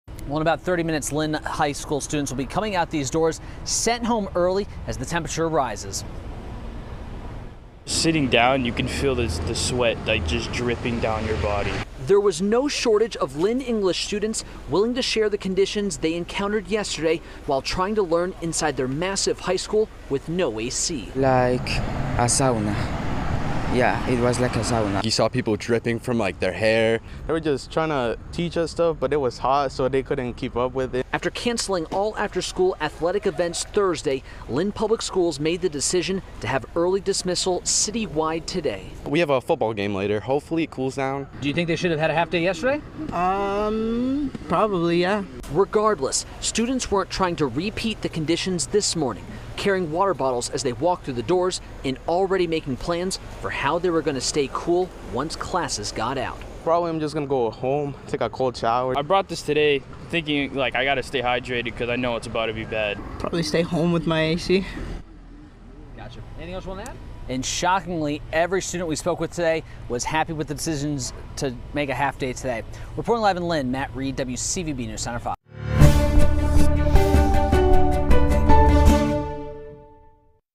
Schools dismiss early as temperatures rise
Several school districts dismissed early Friday as the temperatures across the state soared into the 90s and 80s in some communities.